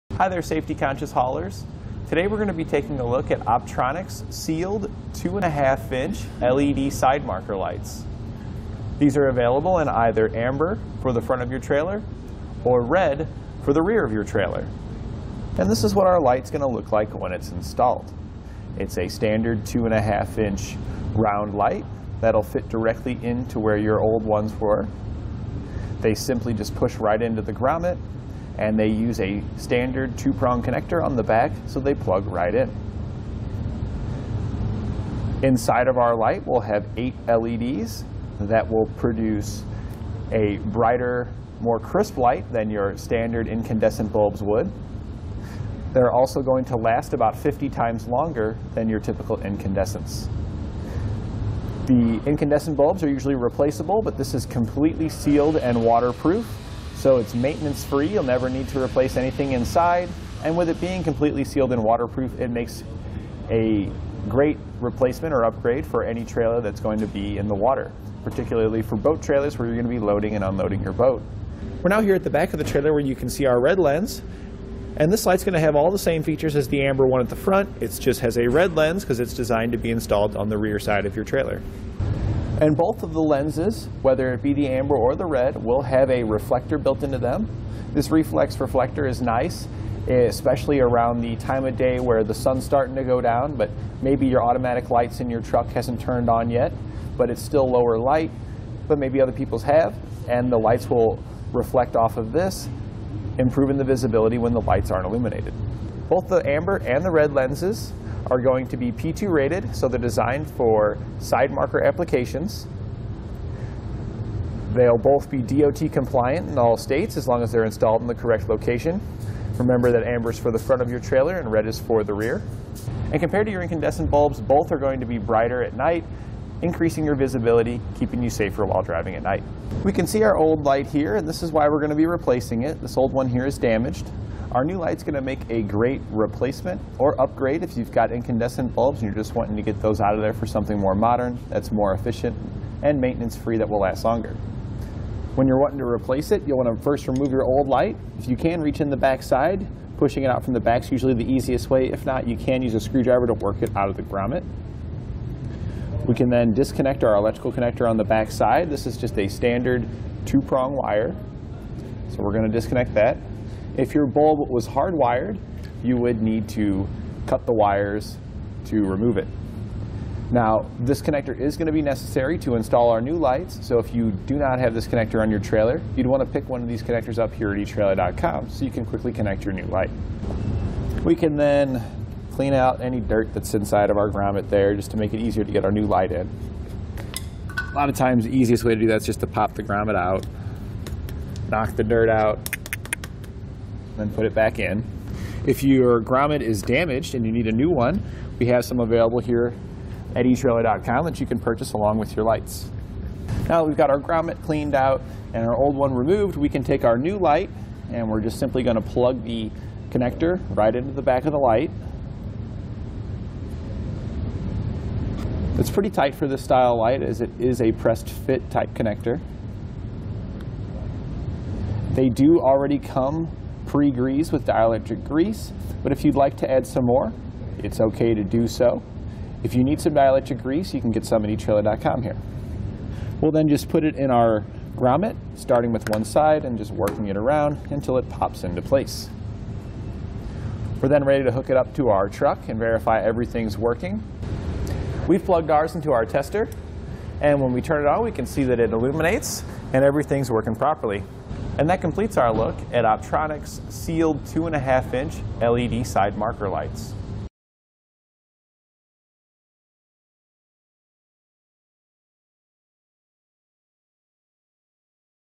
Optronics LED Trailer Clearance or Side Marker Light with Reflex Reflector Review and Installation
Hi there safety conscious haulers. Today we're going to be taking a look at Optronics Sealed 2-1/2" LED Side Marker Lights. These are available in either amber for the front of your trailer or red for the rear of your trailer. And this is what our light's going to look like when it's installed. It's a standard 2-1/2" round light that'll fit directly into where your old ones were. They simply just push right into the grommet and they use a standard two-prong connector on the back so they plug right in.

Inside of our light we'll have eight LEDs that will produce a brighter, more crisp light than your standard incandescent bulbs would.They're also going to last about 50 times longer than your typical incandescents. The incandescent bulbs are usually replaceable, but this is completely sealed and waterproof, so it's maintenance free. You'll never need to replace anything inside. And with it being completely sealed and waterproof it makes a great replacement or upgrade for any trailer that's going to be in the water, particularly for boat trailers where you're going to be loading and unloading your boat.We're now here at the back of the trailer where you can see our red lens and this light's going to have all the same features as the amber one at the front. It's just has a red lens because it's designed to be installed on the rear side of your trailer.

And both of the lenses, whether it be the amber or the red, we'll have a reflector built into them. This reflex reflector is nice, especially around the time of day where the sun's starting to go down, but maybe your automatic lights in your truck hasn't turned on yet, but it's still lower light, but maybe other people's have and the lights will reflect off of this improving the visibility when the lights are illuminated.Both the amber and the red lenses are going to be PT rated, so they're designed for side marker applications. They'll both be DOT compliant in all states as long as they're installed in the correct location. Remember that amber's for the front of your trailer and red is for the rear and compared to your incandescent bulbs, both are going to be brighter at night, increasing your visibility, keeping you safe for while driving at night.We can see our old light here and this is why we're going to be replacing it. This old one here is damaged.

Our new light's going to make a great replacement or upgrade, if you've got incandescent bulbs and you're just wanting to get those out of there for something more modern, that's more efficient and maintenance free, that will last longer. When you're wanting to replace it, you'll want to first remove your old light. If you can, reach in the back side. Pushing it out from the back's usually the easiest way, if not, you can use a screwdriver to work it out of the grommet.We can then disconnect our electrical connector on the back side. This is just a standard two-prong wire.

We're going to disconnect that. If your bulb was hardwired, you would need to cut the wires to remove it. Now, this connector is going to be necessary to install our new lights, so if you do not have this connector on your trailer, you'd want to pick one of these connectors up here at etrailer.com so you can connect your new light.We can then clean out any dirt that's inside of our grommet there just to make it easier to get our new light in. A lot of times the easiest way to do that is just to pop the grommet out, knock the dirt out and then put it back in. If your grommet is damaged and you need a new one, we have some available here at etrailer.com that you can purchase along with your lights. Now that we've got our grommet cleaned out and our old one removed, we can take our new light and we're just simply going to plug the connector right into the back of the light.It's pretty tight for this style light as it is a pressed fit type connector. They do already come pre-greased with dielectric grease, but if you'd like to add some more, it's okay to do so. If you need some dielectric grease, you can get some at etrailer.com here. We'll then just put it in our grommet starting with one side and just working it around until it pops into place. Then ready to hook it up to our truck and verify everything's working. We plugged ours